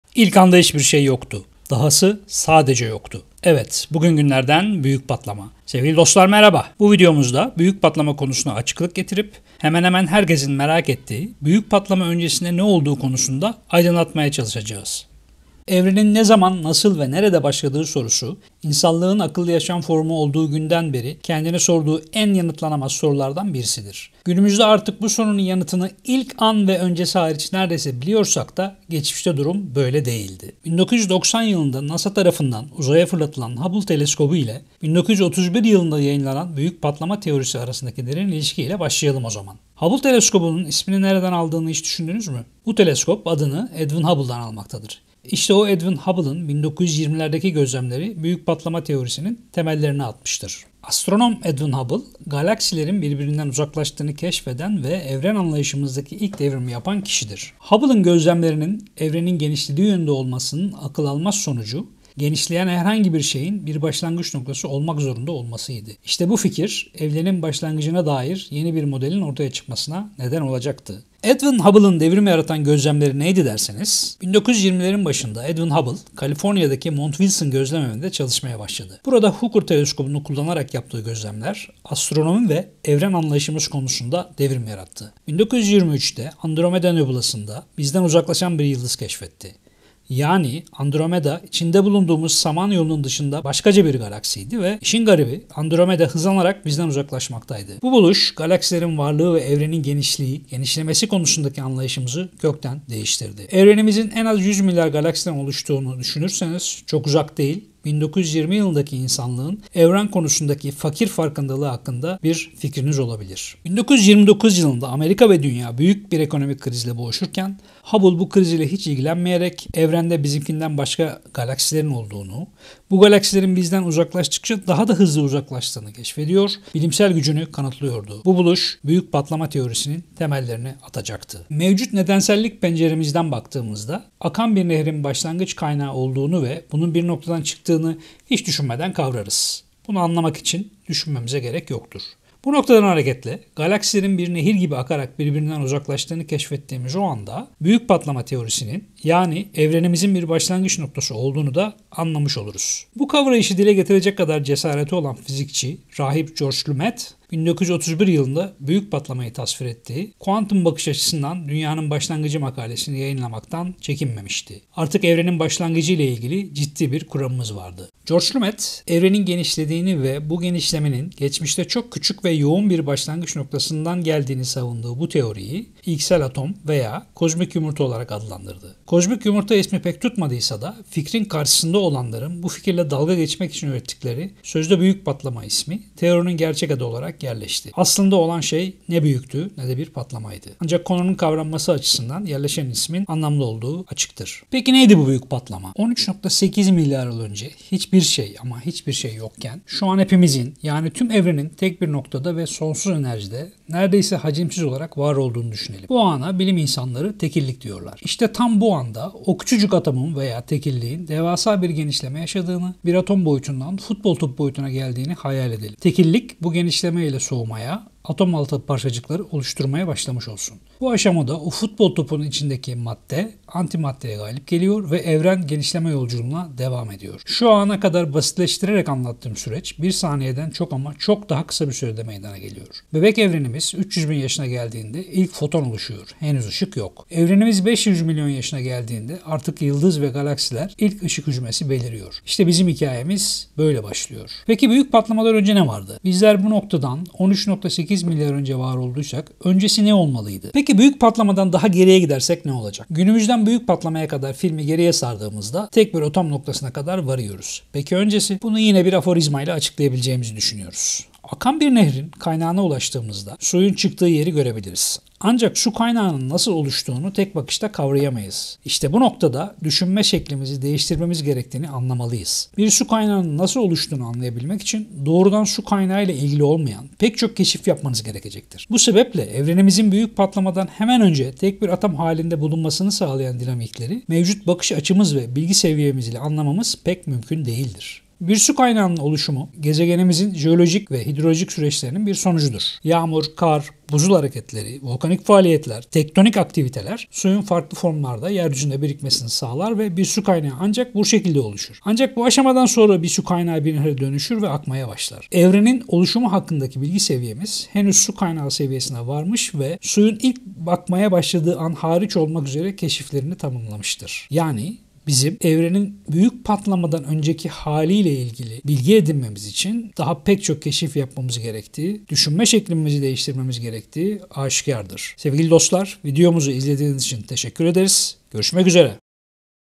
Büyük patlamadan önce ne vardı?
Büyük Patlama'dan önce ne vardı sorusu, evrenin başlangıcına dair en büyük gizemlerden biridir. Kozmolojiye göre, zaman ve uzay Büyük Patlama ile başladı, bu yüzden "önce" kavramı biraz yanıltıcı olabilir. Ancak bazı teoriler, Büyük Patlama'dan önce bir "kozmik yumurta" veya "tekillik" olduğunu öne sürer. Bu durumda, evren aşırı yoğun ve sıcak bir noktada sıkışmıştı. Diğer teoriler, sürekli genişleyen ve büzülen bir evren modeli olan "Büyük Çöküş" veya çoklu evrenler teorisini öne sürer. Kesin cevabı bilmesek de, bu sorular bilimin sınırlarını zorlamaya devam ediyor.